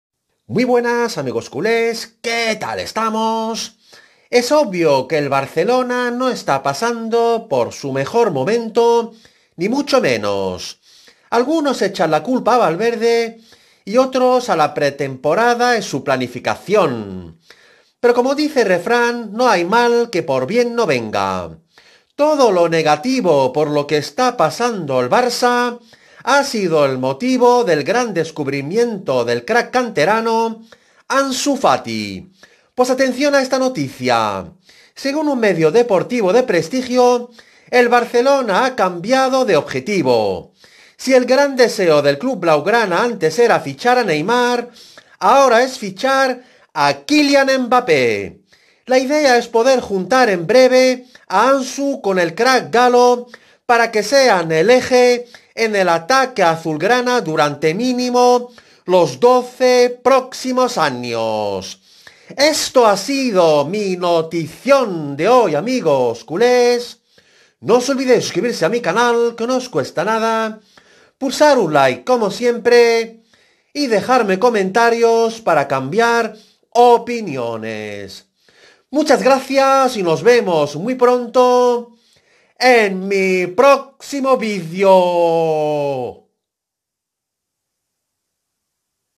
ÚLTIMA HORA: ¡¡FICHAJAZO DEL SIGLO DEL BARCELONA PARA ACOMPAÑAR A ANSU FATI!!
El Barcelona firma un acuerdo con un crack mundial para acompañar a Ansu Fati y garantizar el mejor ataque del mundo durante los próximos doce años